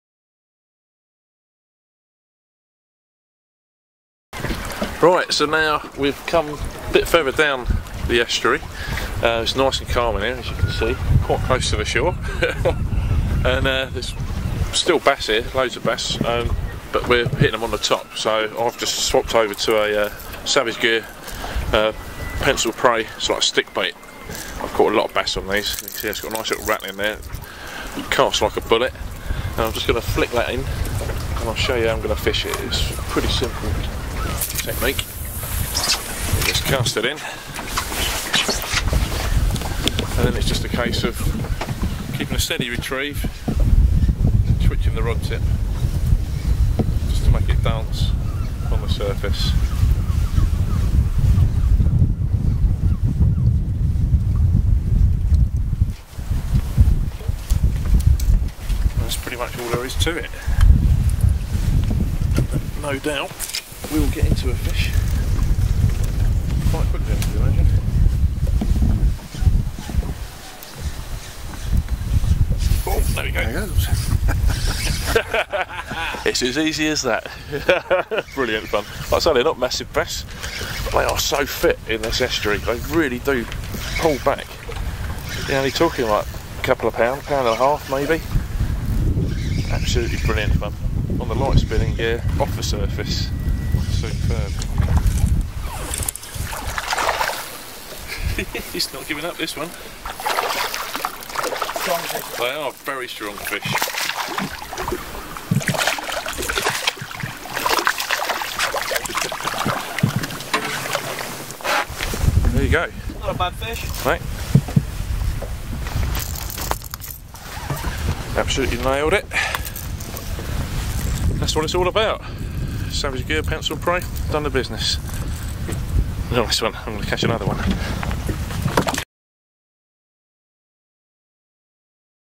Bass On The Savage Gear Pencil Prey Lure
Dave Barham demonstrates how to catch bass off the surface on the Savage Gear Pencil Prey stickbait lure.